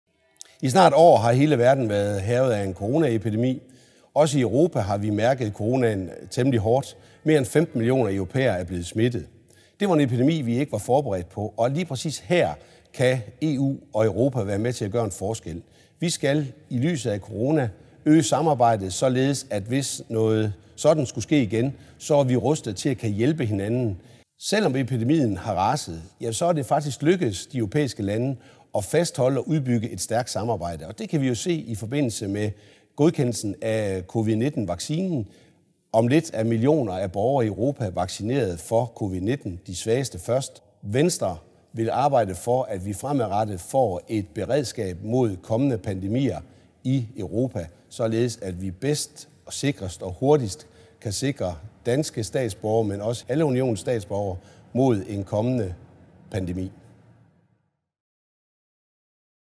Hvorfor EU-samarbejdet bringer Danmark godt igennem Coronakrisen 🌍🦠
Coronakrisen kender ikke til landegrænser. Derfor er et stærkt samarbejde i EU et af vores bedste værnemidler her i Danmark. Læs mere om EU og EU's håndtering af corona-virussen på

Medvirkende: MEP Søren Gade (V)